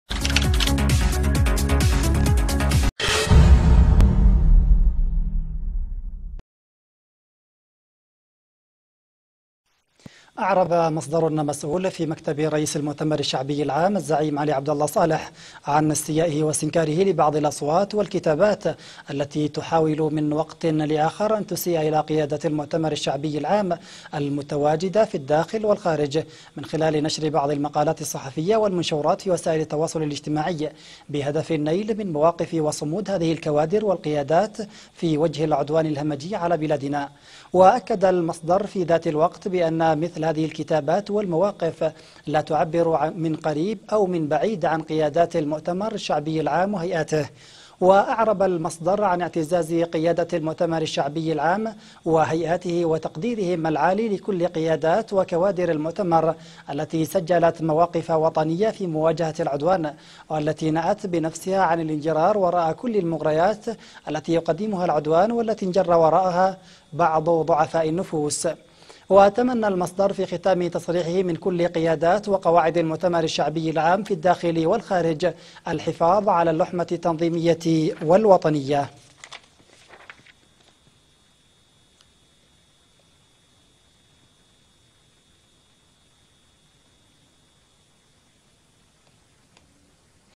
مصدر بمكتب رئيس المؤتمر الشعبي العام يستنكر اساءات متكررة ضد قيادات في الداخل والخارج
قناة اليمن اليوم : رؤى تجمعنا.. للاشتراك على التلغرام